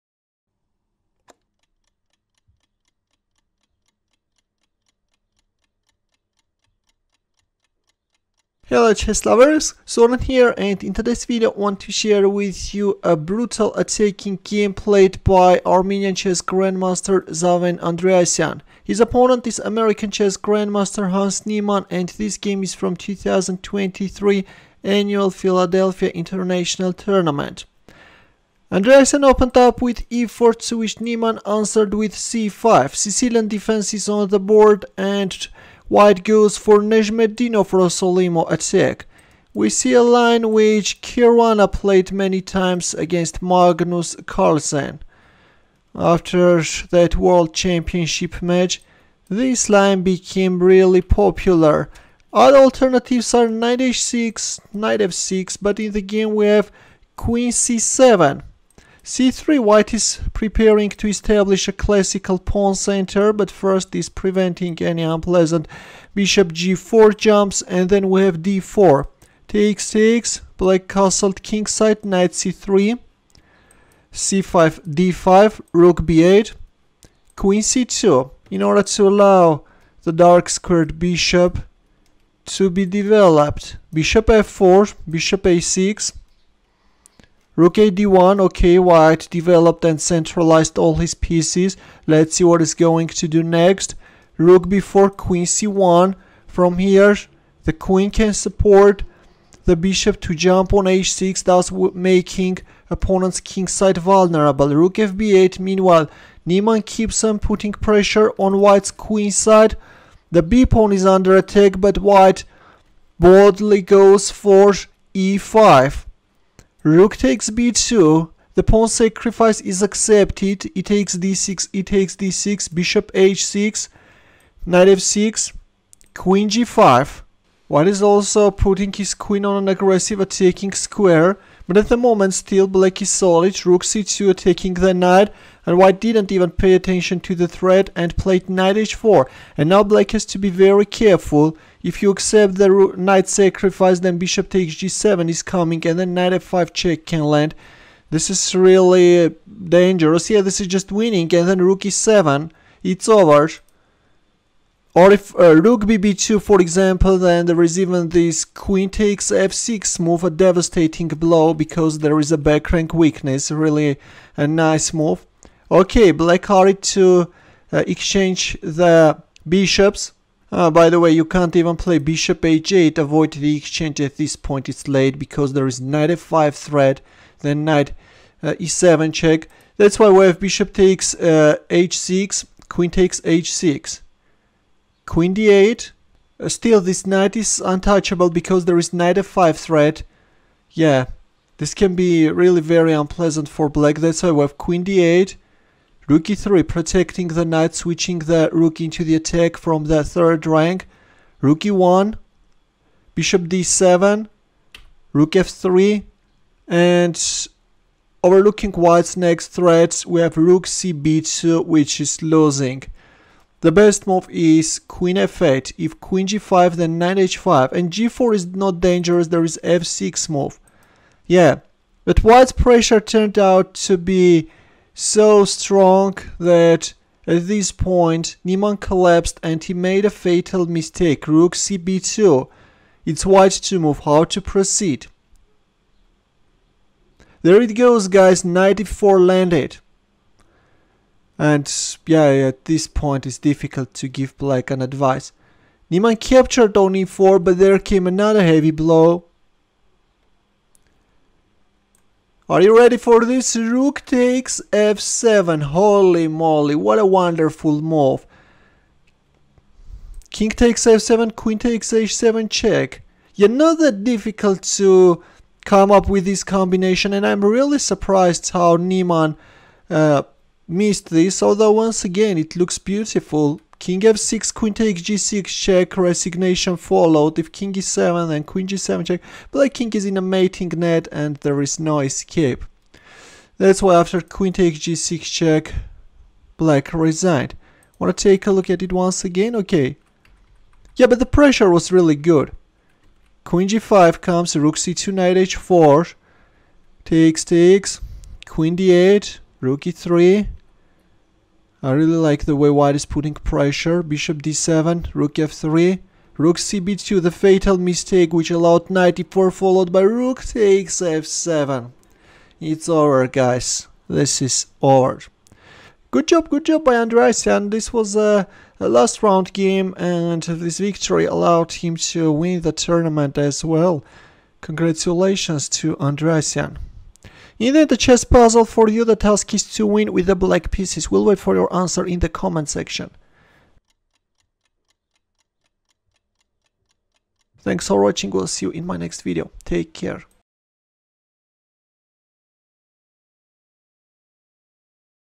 Niemann Overlooks A Brutal Sacrifice And Goes Down
This is Andriasian vs Niemann chess game analysis!
Zaven Andriasian vs Hans Niemann
16th Annual Philadelphia International
B31 Sicilian Defense: Nyezhmetdinov-Rossolimo Attack
1. e4 c5 2. Nf3 Nc6 3. Bb5 g6 4. Bxc6 bxc6 5. O-O Bg7 6. Re1 Qc7 7. c3 d6 8. h3 Nf6 9. d4 cxd4 10. cxd4 O-O 11. Nc3 c5 12. d5 Rb8 13. Qc2 Nd7 14. Bf4 Ba6 15.Rad1 Rb4 16. Qc1 Rfb8 17. e5 Rxb2 18. exd6 exd6 19. Bh6 Nf6 20. Qg5 Rc2 21. Nh4 Bxh6 22. Qxh6 Qd8 23. Re3 Bc8 24. Rde1 Bd7 25. Rf3 Rcb2 26. Ne4 Nxe4 27. Rxf7 Kxf7 28. Qxh7+ Kf6 29. Qxg6+
 1-0